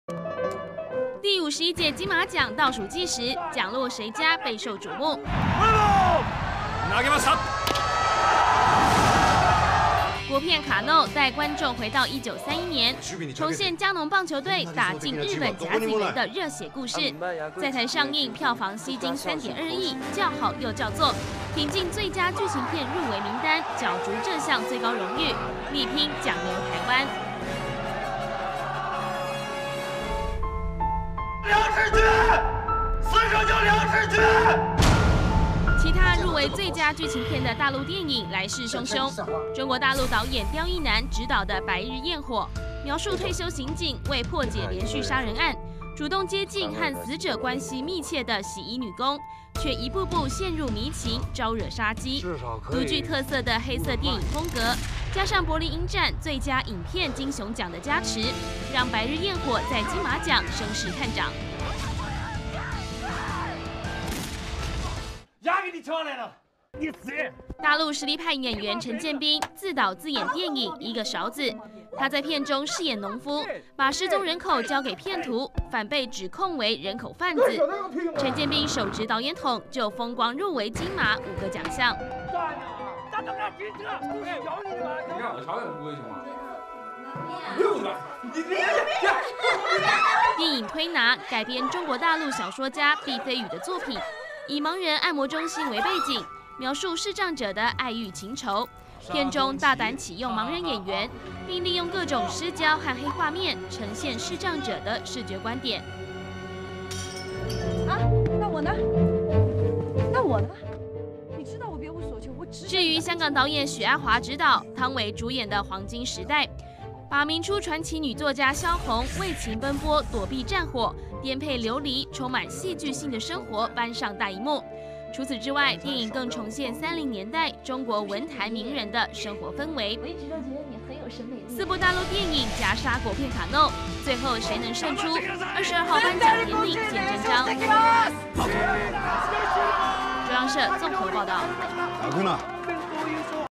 金馬劇情片 KANO拚4陸片
第51屆金馬獎倒數計時，獎落誰家備受矚目。國片《KANO》帶觀眾回到1931年，重現嘉農棒球隊打進日本甲子園的熱血故事。在臺上映票房吸金3.2億，叫好又叫座，挺進最佳劇情片入圍名單，角逐這項最高榮譽，力拚獎留臺灣。